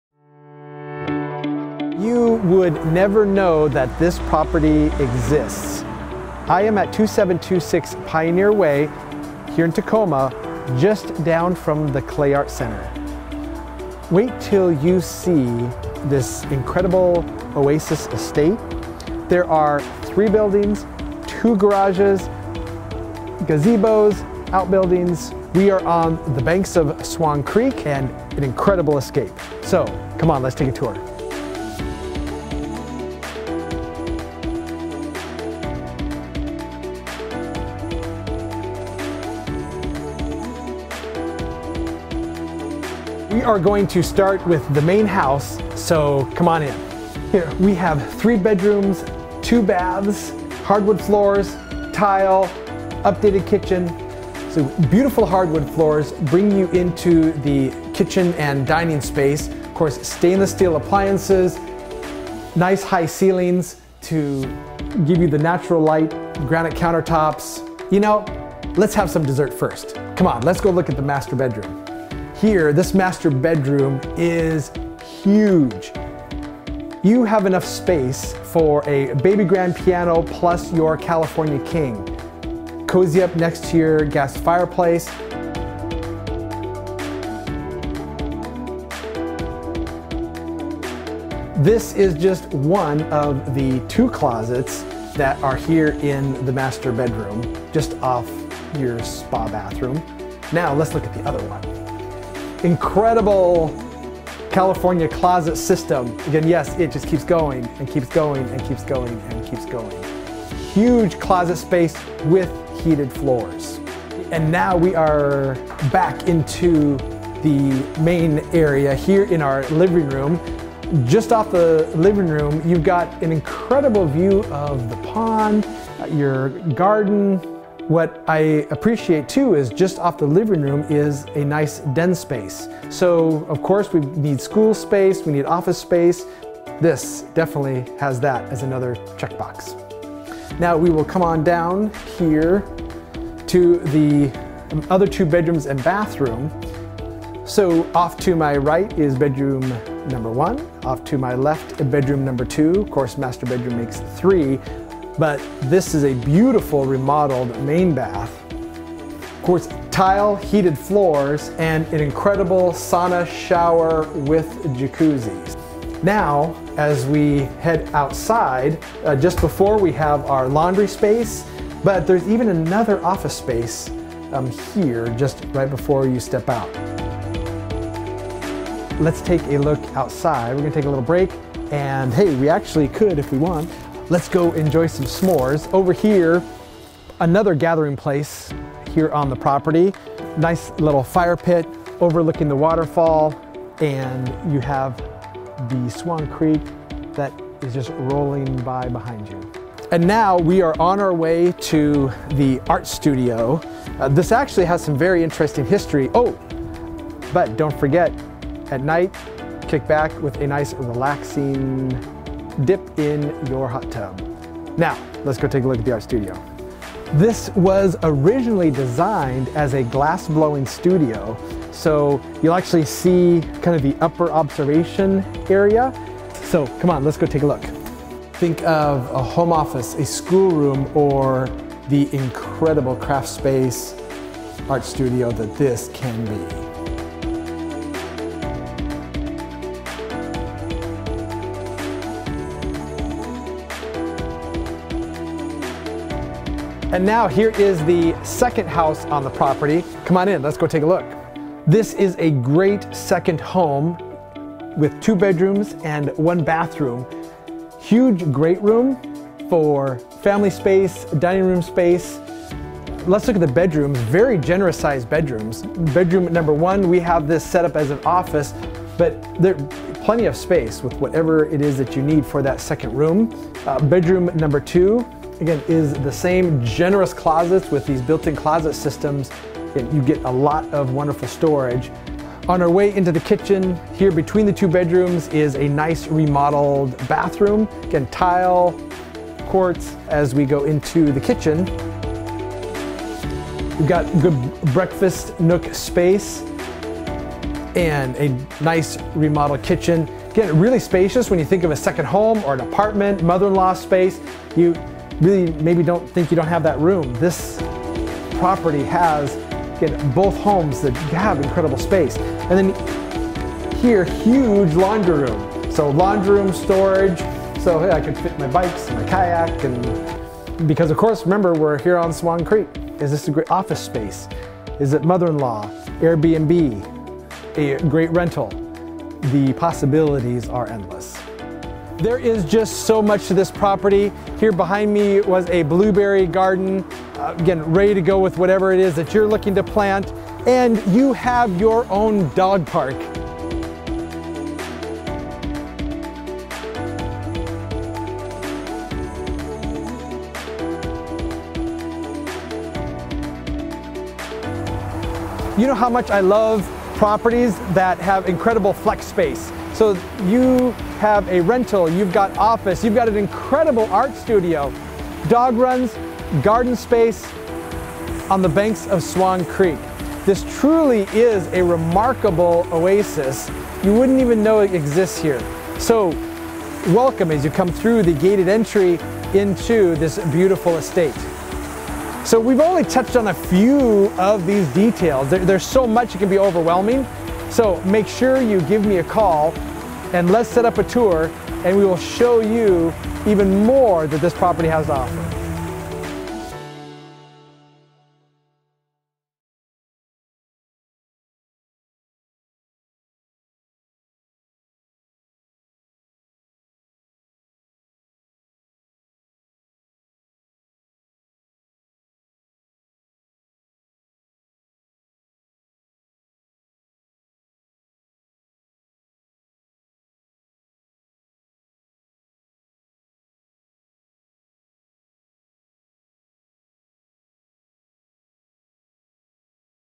Sold - 2726 Pioneer Way E, Tacoma, WA (video tour)
MLS# 1770622

On the banks of Swan Creek through the gated entry is an amazing hideaway estate with 2 homes, garage/shop, & studio, huge dog run, koi pond & waterfall. Main house is over 2000 sq ft, 3 bedrooms/2 bathrooms with updated kitchen, granite counters and gas stove. Hardwoods and tile. Incredible master suite with 2 closets, California Closets system, floor heating, gas fireplace too. Room for a baby grand piano. Your living room will relax you with a waterfall view that includes office/den space. 2nd home is 1200 sqft, 2 bedrooms/1 bath with laundry. Don't forget about the art studio or office & garages. Come and stroll next to the creek, relax in the gazebo and hot tub, sit next to the waterfall... this is a true retreat! Must see!